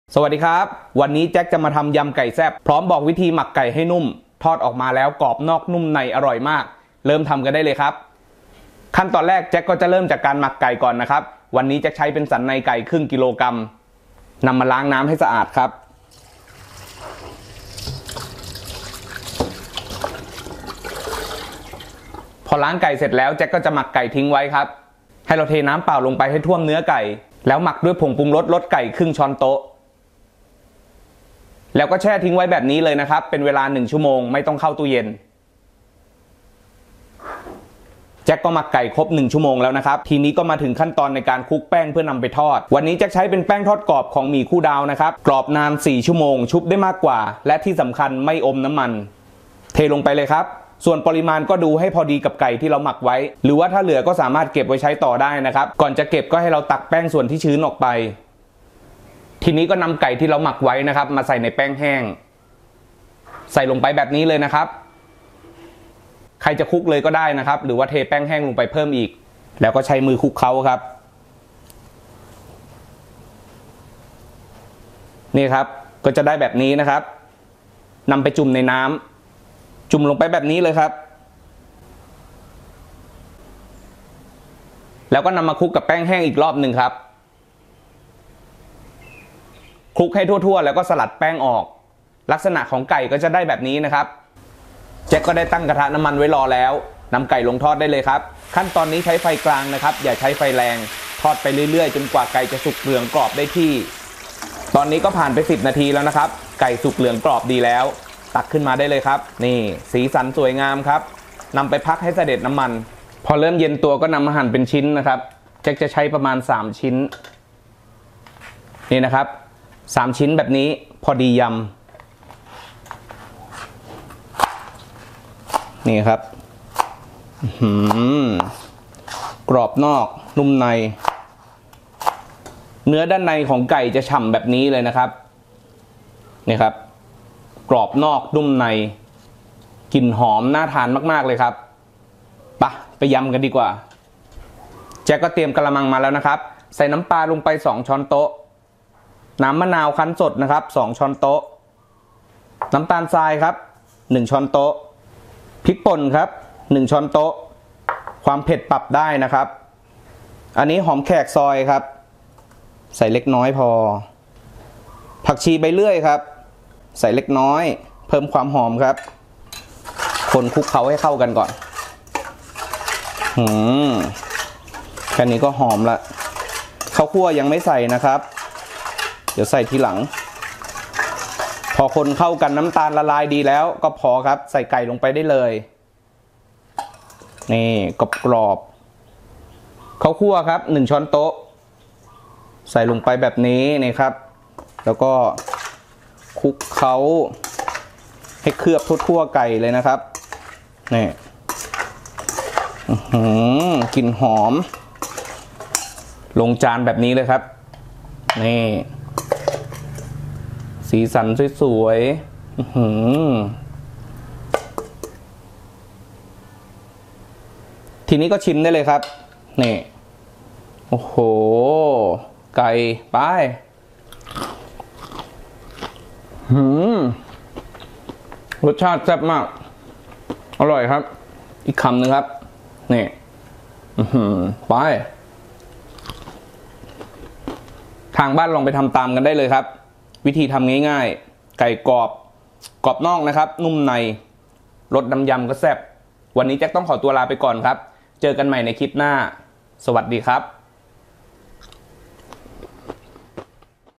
ยำไก่แซ่บ ไก่กรอบ ทอดง่าย ไม่อมน้ำมัน
สันในไก่ 1/2 กิโลกรัม
ผงปรุงรสรสไก่ 1/2 ช้อนโต๊ะ
แป้งทอดกรอบ ตราหมีคู่ดาว
ข้าวคั่ว 1 ช้อนโต๊ะ
หอมแดงซอย ใส่เล็กน้อย
ผักชีใบเรื่อยซอย ใส่เล็กน้อย

หม้อน่ารักราคาถูก
ที่กรีดผักบุ้ง มีดซอยผักบุ้ง มีดปลอกผักบุ้ง ผักบุ้ง ผักบุ้งฝอย มีดตัดผักบุ้ง มีดปอกผักบุ้ง